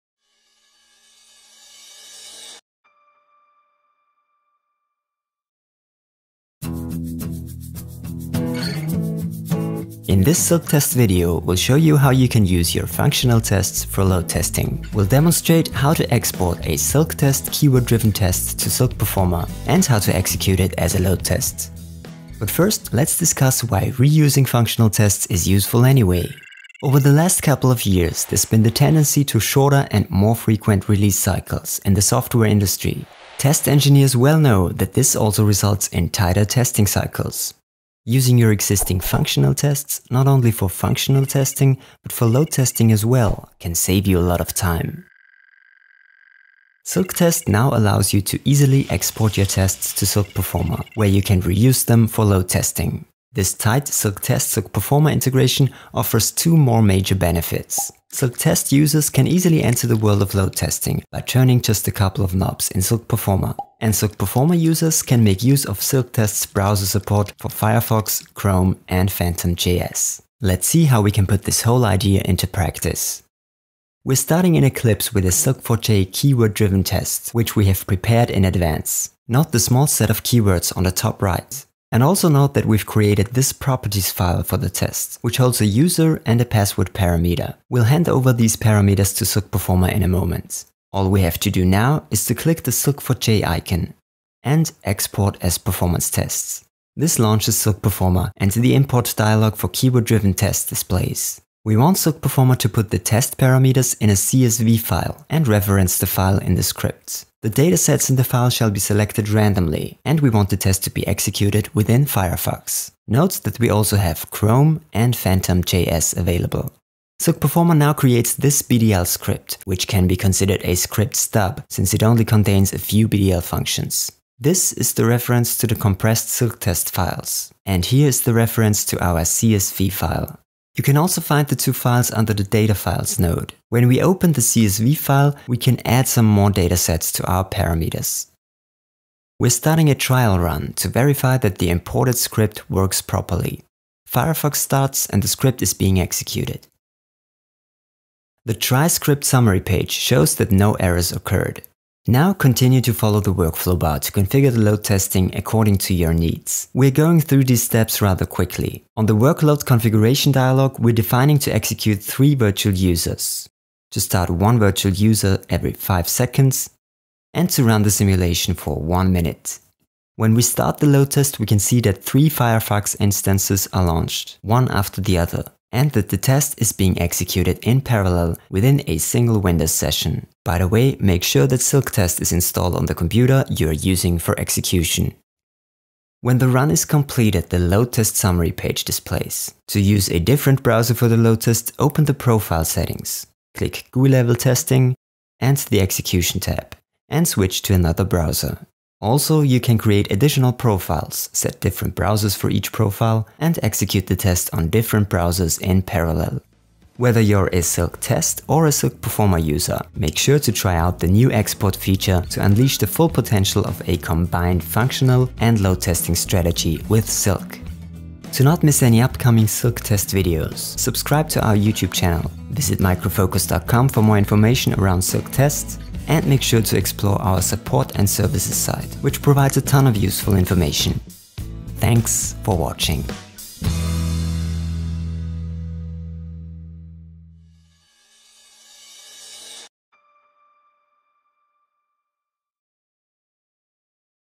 Silk Test - Functional Tests for Load Testing
- Watch this video to see how you can use your functional tests for load testing. First, we’re going to discuss the benefits of a combined functional and load testing strategy using Silk solutions. And then we’ll demonstrate, based on a sample project, how to export a Silk Test keyword-driven test to Silk Performer and how to execute it as a load test.